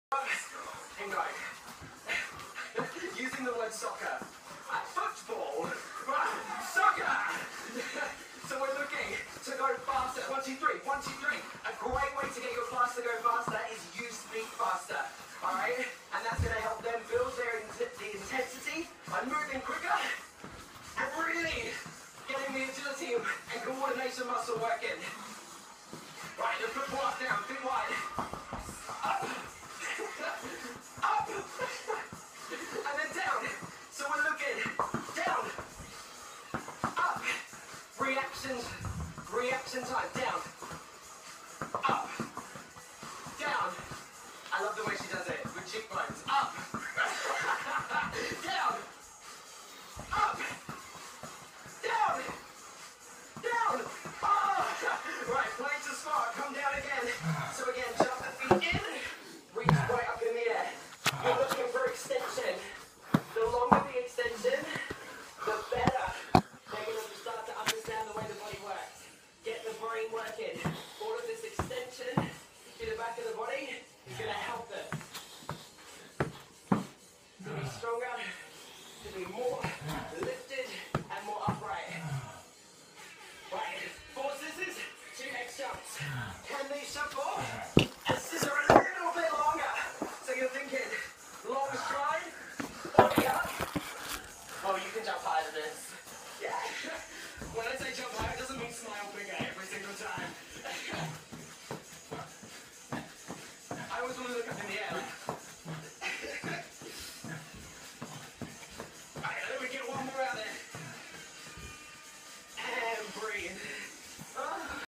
INSANITY Round 13-Block 3. Low impact (modified, no jumping ) moves--1-2-3 Soccer Juggle, Football Up/Down, Plank to Squat, 4 Scissors/2 X-Jumps